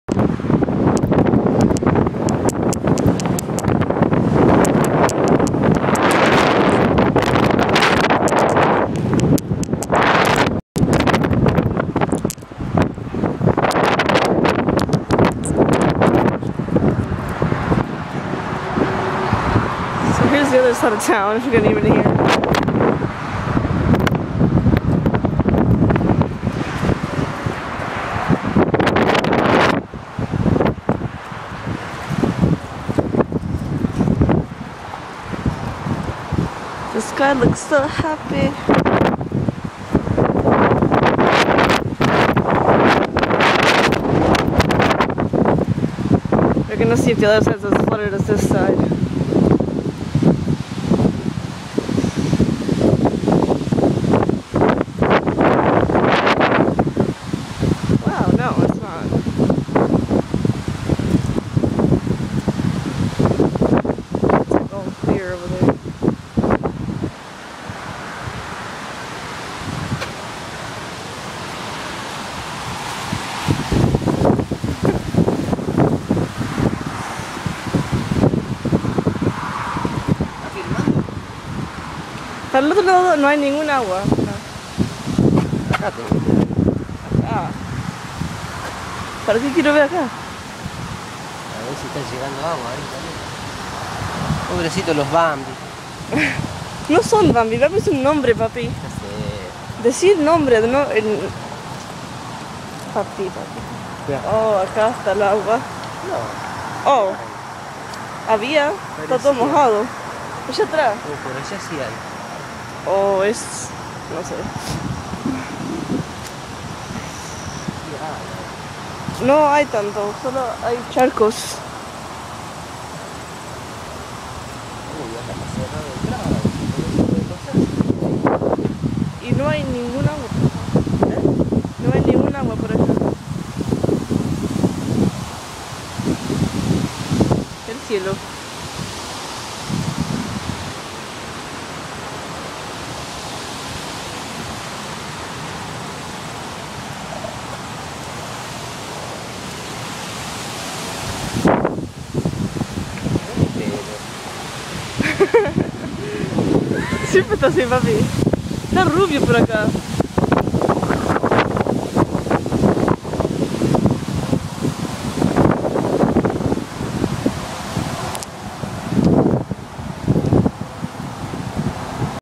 Checking to see if the other side of town was as flooded
Saddle brook new jersey